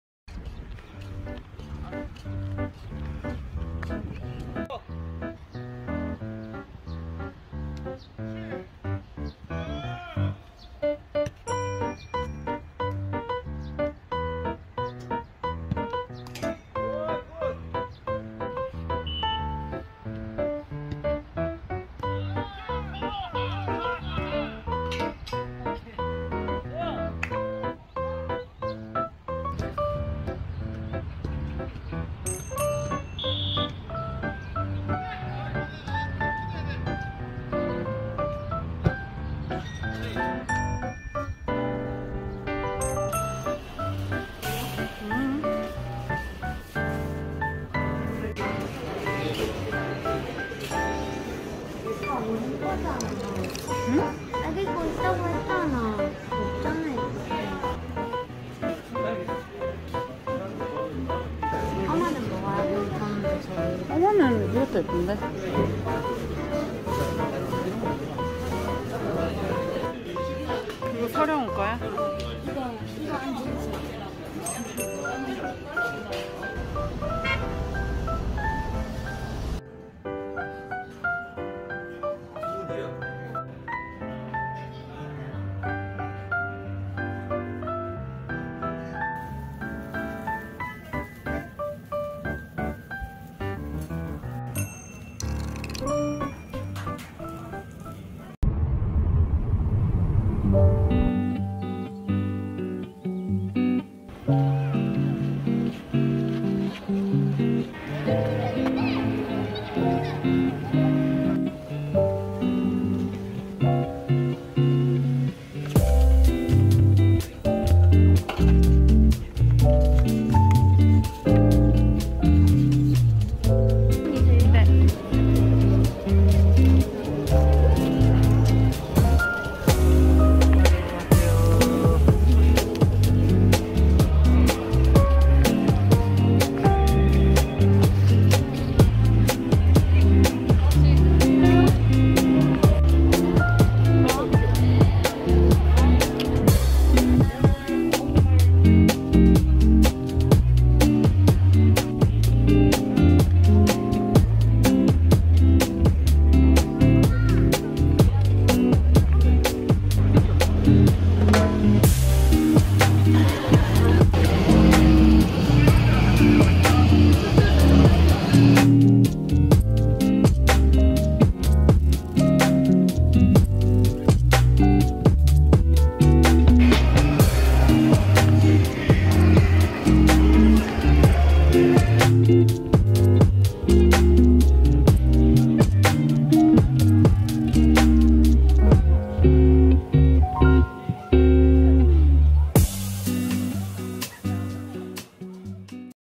[축구하는 준수] 축구로 시작해서 축구로 끝나는 주말 브이로그⚽️ /울산현대vs인천UTD/
이번 영상은 축구하는 준수 주말 브이로그 입니다

축구로 시작해서 축구로 끝나는 브이로그이지만 재미있게 시청해주세요💙